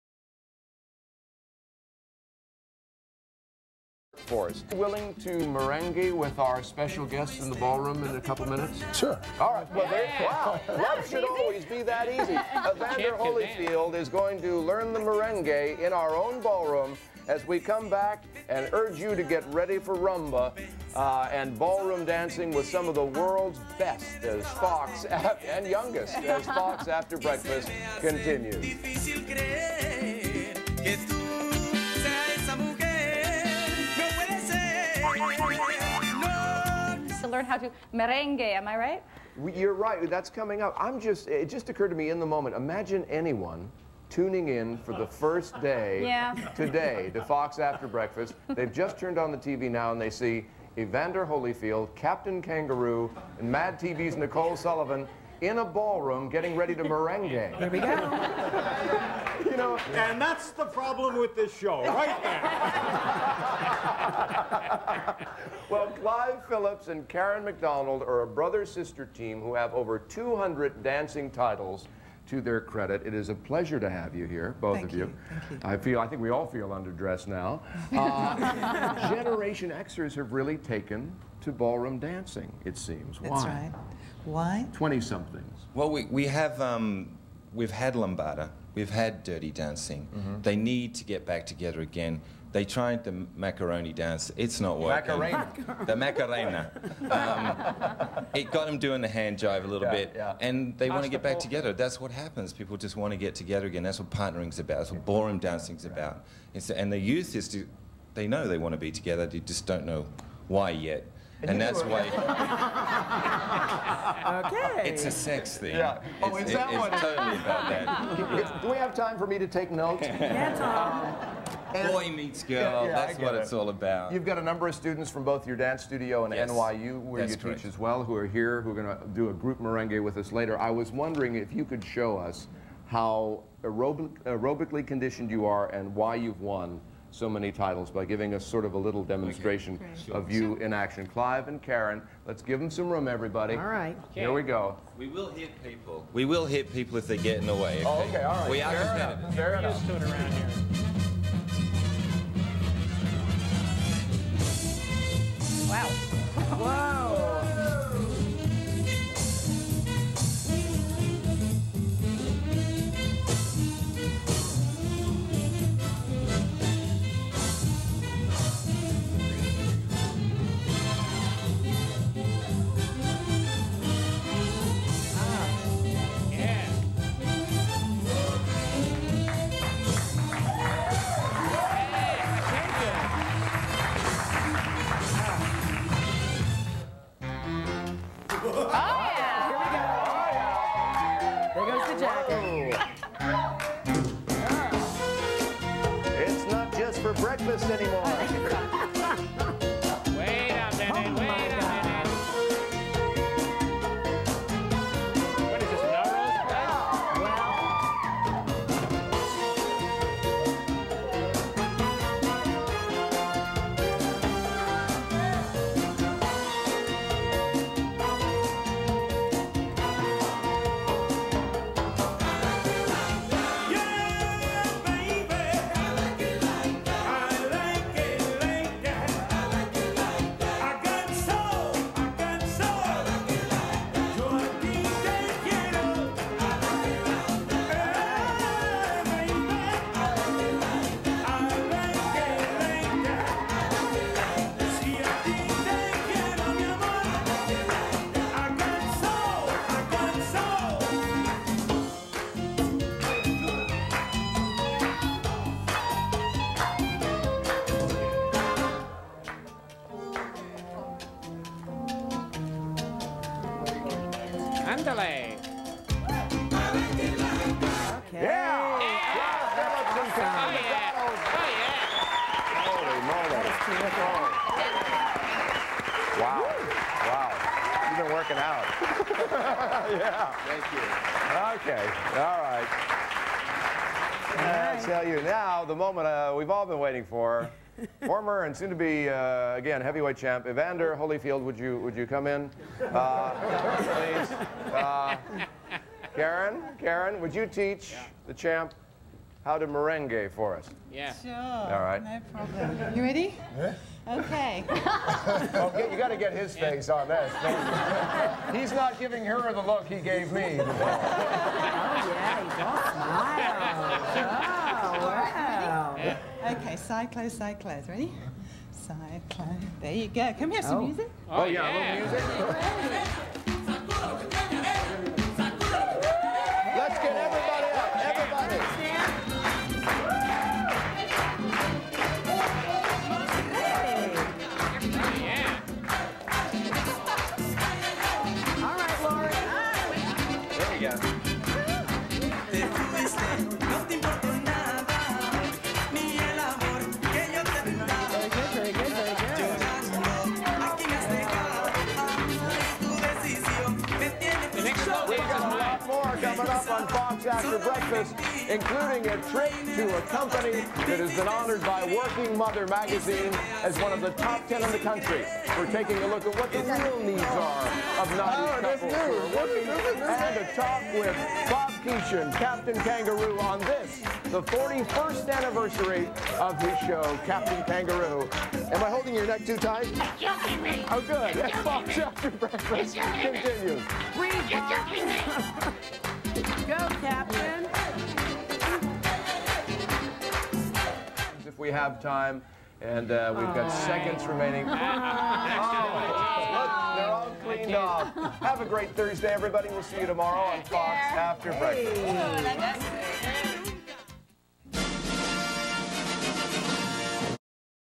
Fox After Breakfast: Clive & Karen
Clive was my first professional dance teacher.   He and his wife had the BEST group classes.   And we also learned a lot from him in private lessons.

Karen is also an awesome dance coach.